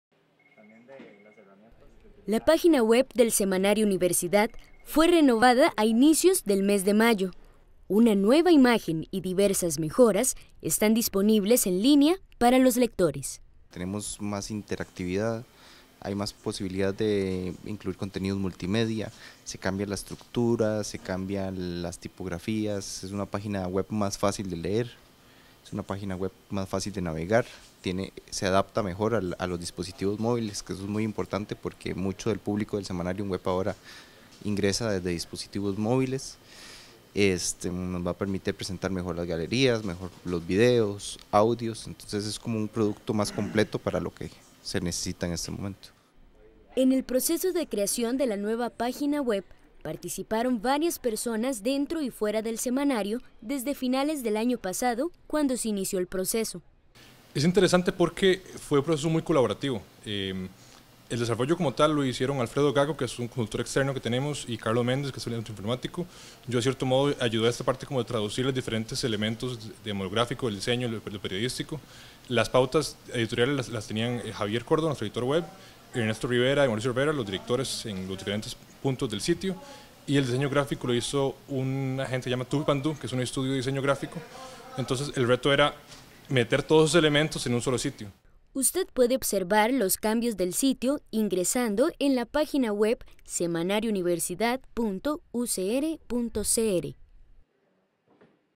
Sitio Web Semanario
El Semanario Universidad estrenó el pasado 6 de mayo su nueva plataforma web, la cual muestra novedades y mejoras para sus lectores por internet.